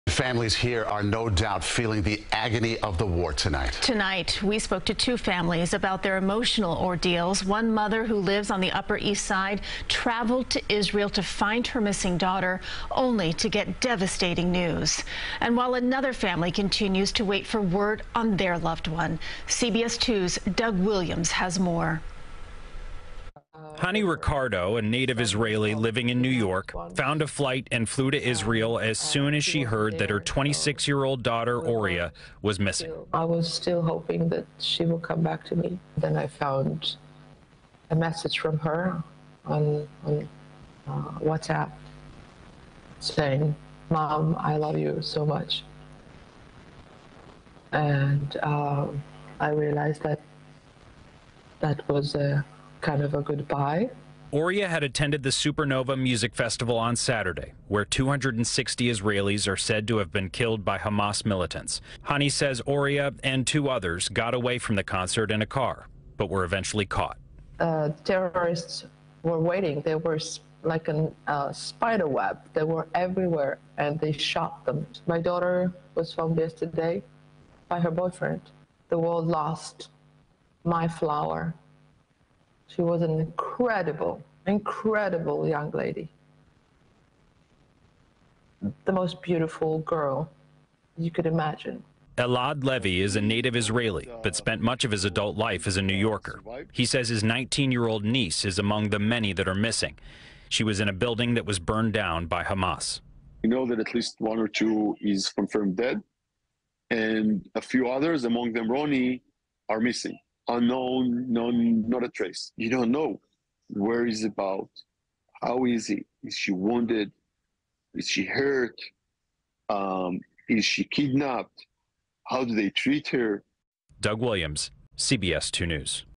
2 families with NYC ties grieving the loss of daughters in Israel
One woman flew from New York to Israel thinking her daughter was alive, only to find out she was killed at a music festival. CBS New York's Doug Williams reports.